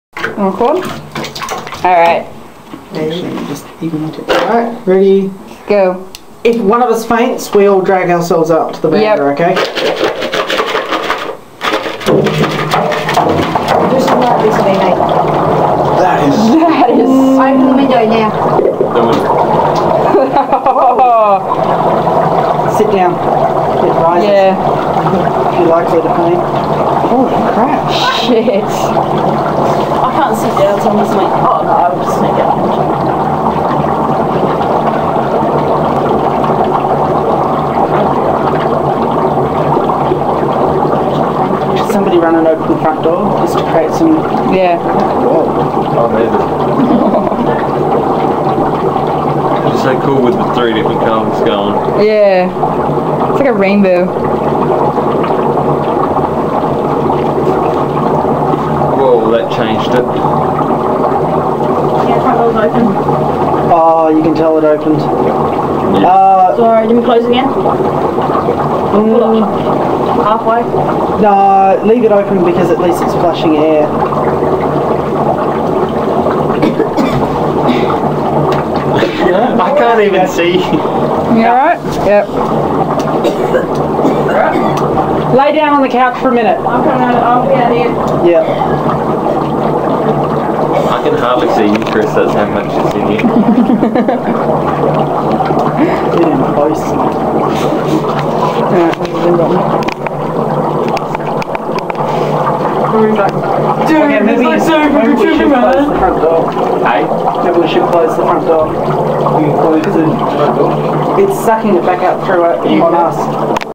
Dry ice, hot water and glowsticks. What more needs to be said!
As said above. The result of combining dry ice, glowsticks and hot water in a large bath tub. Remember that if youre recreating this to do it in a well ventilated area. As you can here, one of us suffered the early stages of asphyxia due to the carbon dioxide emitted. Also, both hot water and dry ice burn! How dumb would you look rocking up to the emergency ward with both heat and cold burns on the one limb!